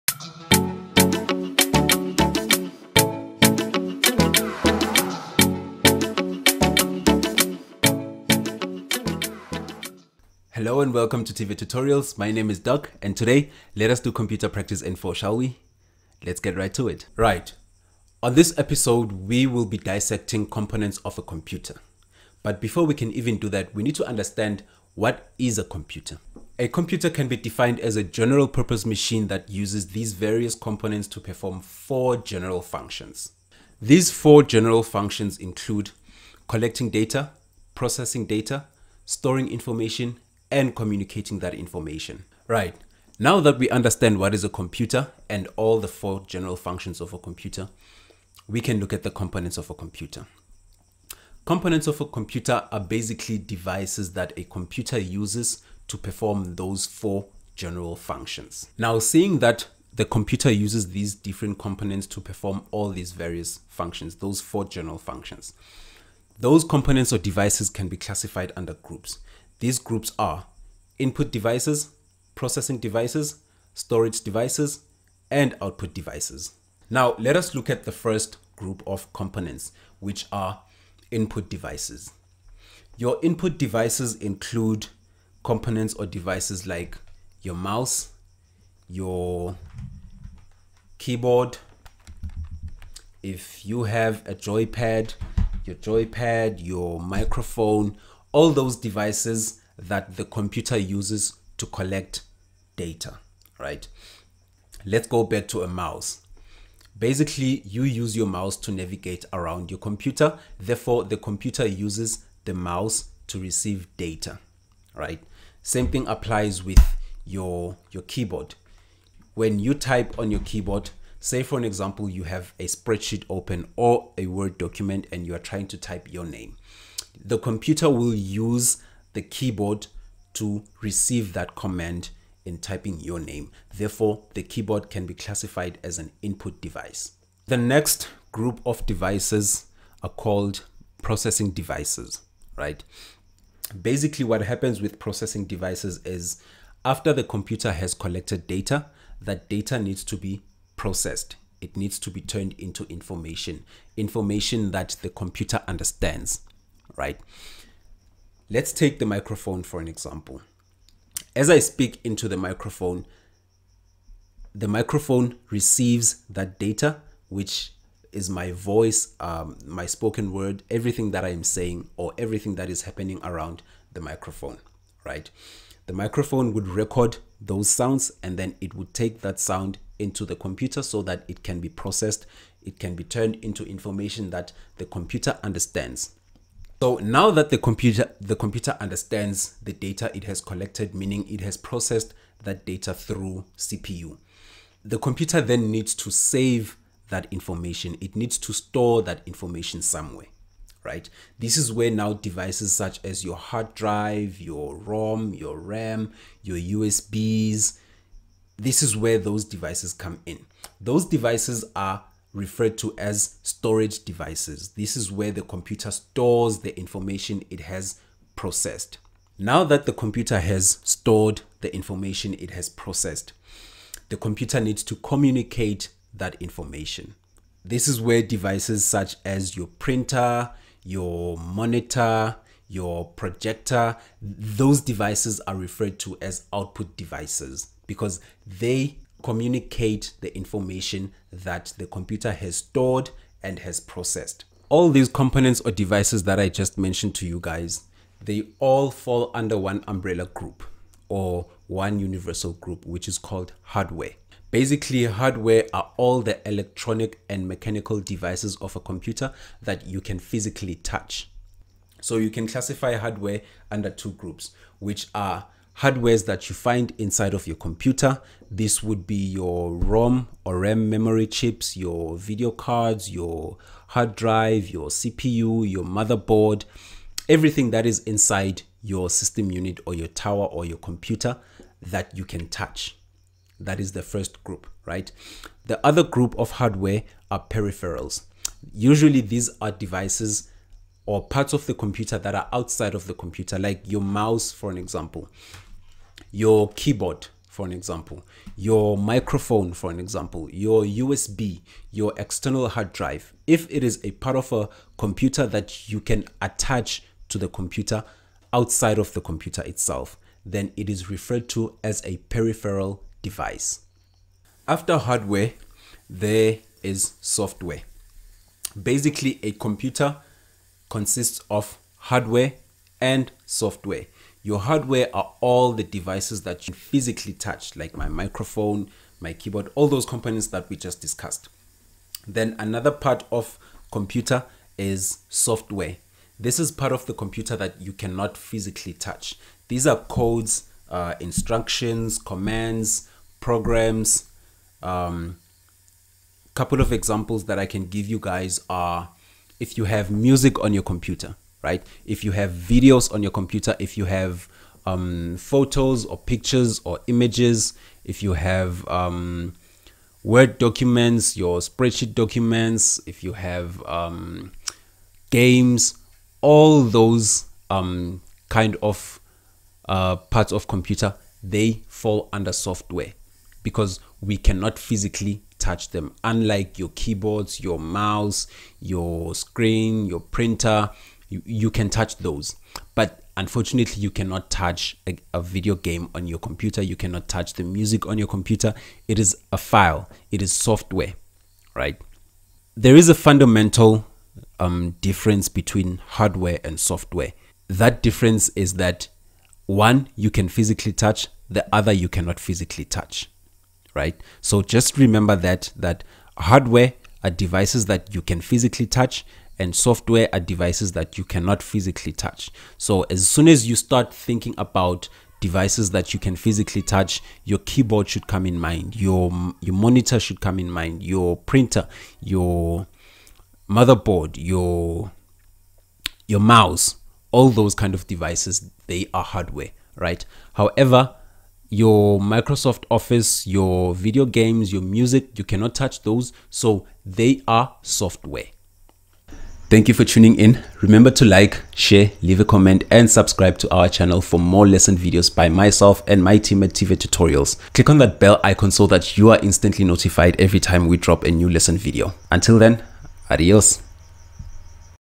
COMPUTER PRACTICE N4: What Is A Personal Computer?
COMPUTER PRACTICE N4

What is a personal computer?

This video is a simple explainer video on what a personal computer is, including all the components of one, any devices that can interact with it, how everything connects.

Join us on this lesson for N4 Business Studies students. Join Our host Doctor Khoza on this exciting lesson.

@TVET Tutorials